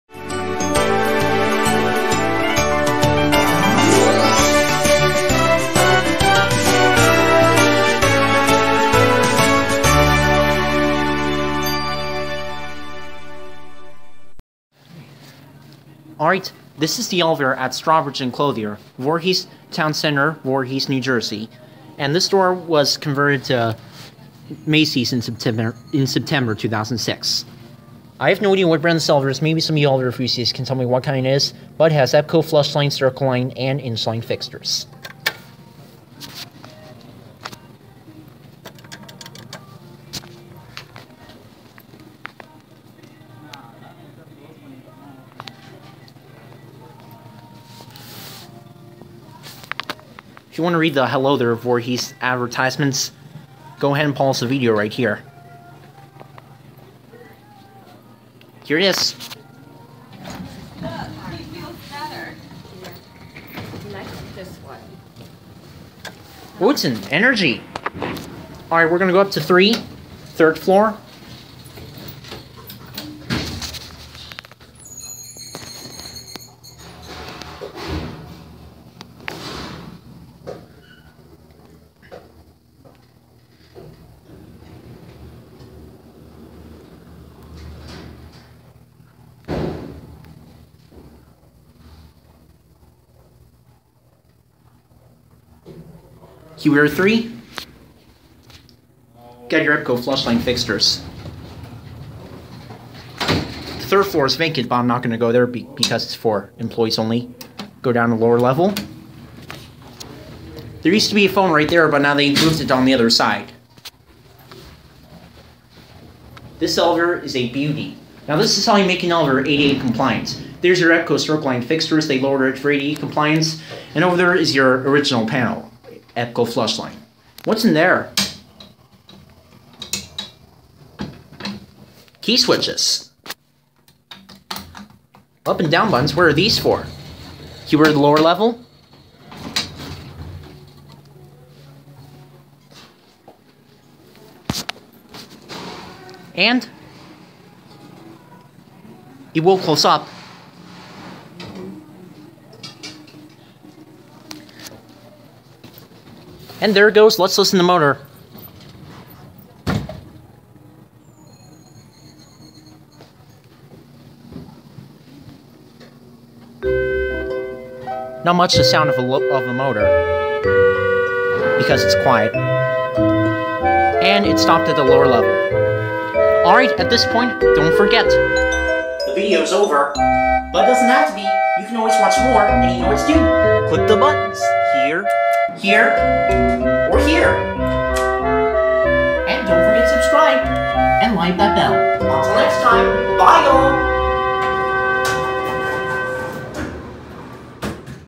Beautiful 1970 Energy Hydraulic elevator @ Strawbridge & Clothier, Echelon Mall, Voorhees Twp NJ
(2-4-17) Here's a flashback to when this elevator was still around. Sadly, it is no longer accessible as this store is currently vacant since Spring 2017. I wonder what's going into that space.

Strawbridge & Clothier: 1970-96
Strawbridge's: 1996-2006
Macy*s: September 9, 2006-March 26, 2017 (Currently Vacant)